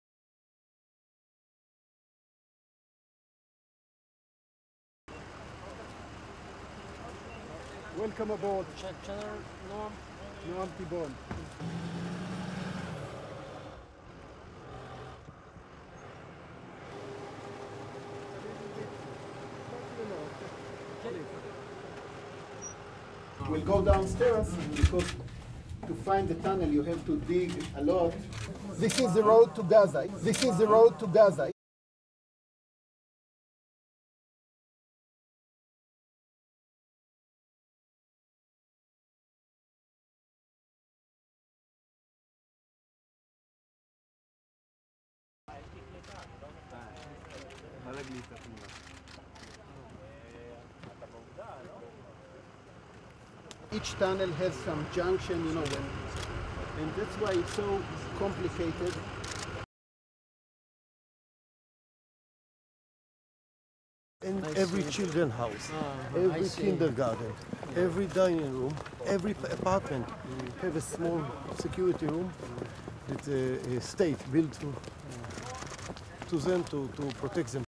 UN Secretary-General, Ban Ki Moon Visits Hamas Terror Tunnel in Southern Israel
On October 14, 2014, the UN Secretary-General, Mr. Ban Ki Moon visited a Hamas terror tunnel in southern Israel. While visiting the tunnel he was briefed on the extent of Hamas' strategic abuse of civilian infrastructure in order to carry out attacks against the civilian communities of southern Israel.

Hamas chose to invest its resources in the construction of terror tunnels rather than the well-being and development of its own population.